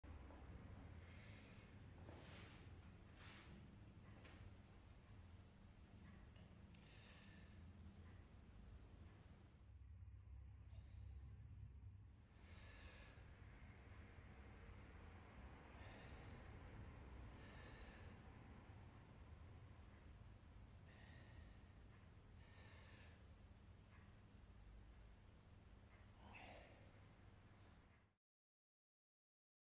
Half Kneeling Shoulder Screw + Hip ER
- Begin by kneeling on the ground in a half-kneeling position, but sit down onto your rear ankle as you rotate your knee out at 90-degrees.
- From here, anchor your elbow inside your front knee as you rotate your shoulders to open your chest as much as possible - you should feel this in your mid back.
- Return to the start position and repeat for the prescribed number of repetitions before doing the same on the other side.